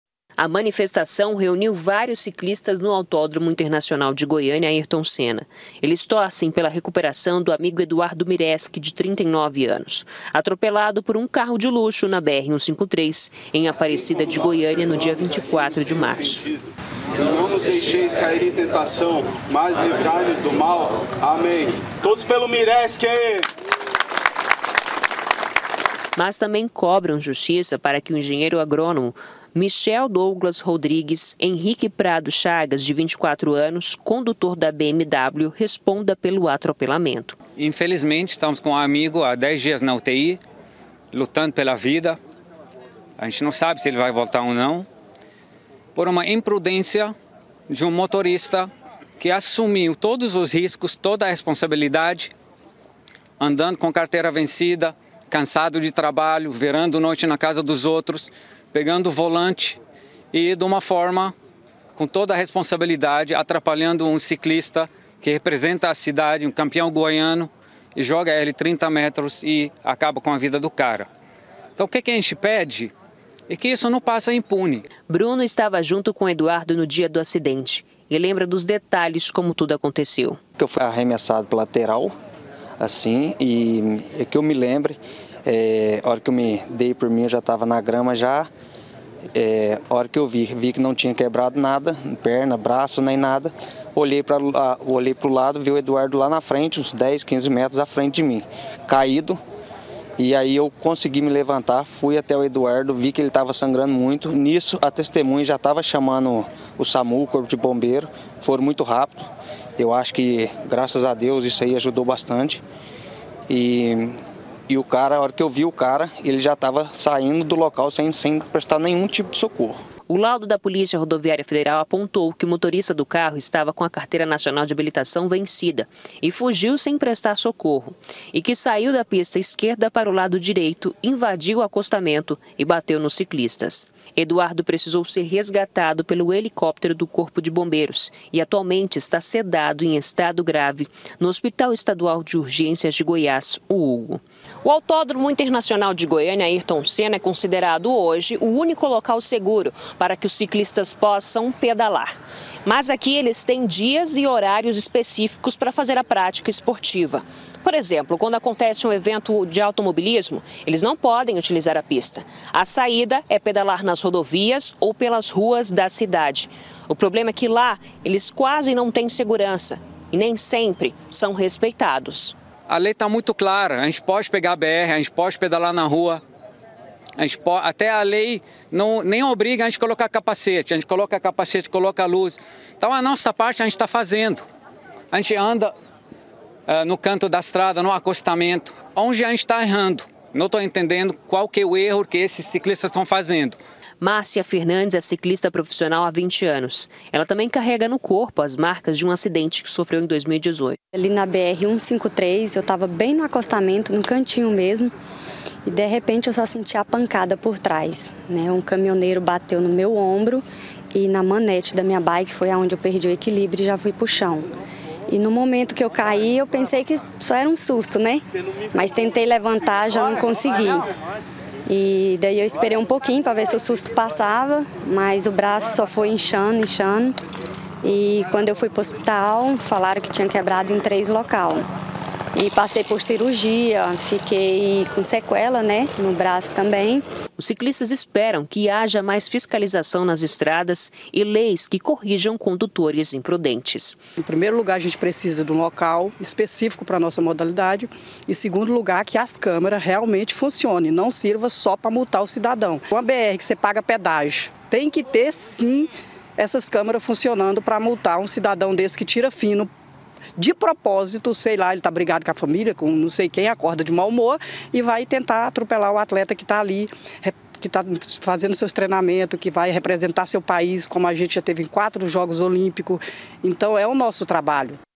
CICLISTA ATROPELADO: AMIGOS FAZEM MANIFESTAÇÃO POR JUSTIÇA
AMIGOS DO CICLISTA ATROPELADO NA BR 153 FIZERAM UMA MANIFESTAÇÃO HOJE DE MANHÃ. EDUARDO MIRESKI, DE 39 ANOS, CONTINUA INTERNADO EM ESTADO GRAVE. ELE E O AMIGO BRUNO MELO ESTAVAM PEDALANDO PELA BR-153, EM APARECIDA DE GOIÂNIA, QUANDO FORAM ATROPELADOS POR UM CARRO DE LUXO.